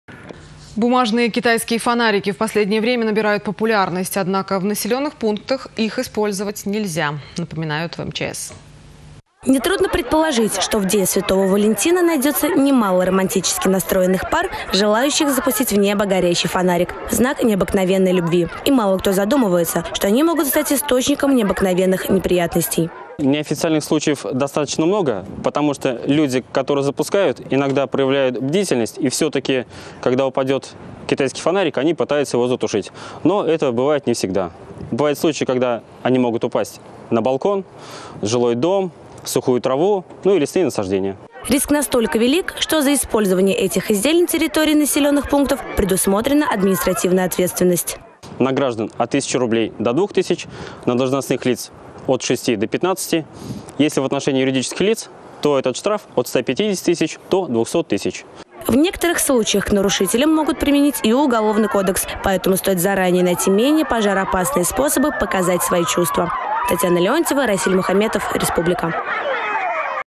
Запрет на фонарики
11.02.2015  Бумажные китайские фонарики в последнее время набирают популярность. Однако в населенных пунктах их использовать нельзя — напоминает МЧС России.